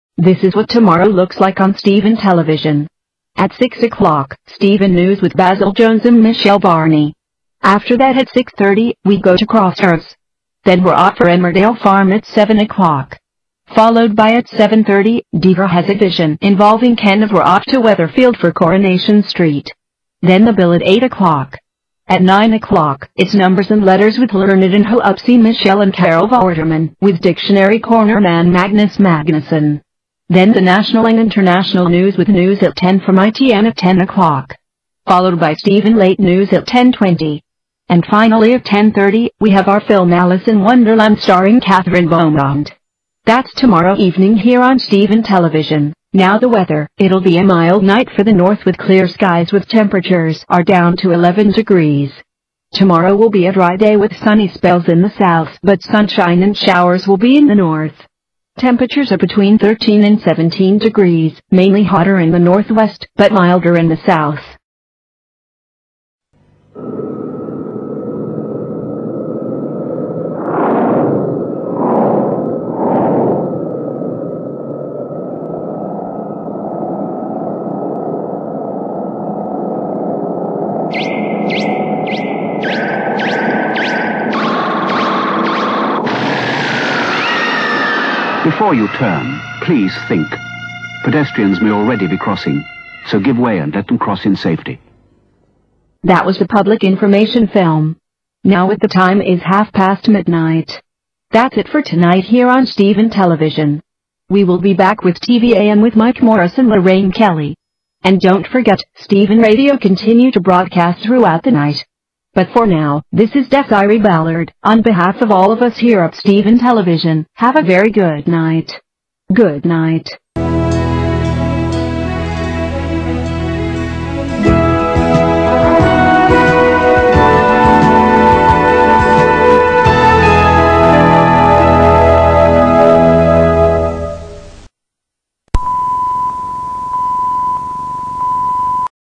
Steven Closedown Thursday 12th March 1987
This is a Steven Closedown from Thursday 12th March 1987. It starts off with the menu, followed by the weather, then a Public Information Film. After that, a clock into the closedown sequence, then the testcard. Please Enjoy! NOTE: Let's hope graphicgrief loves this.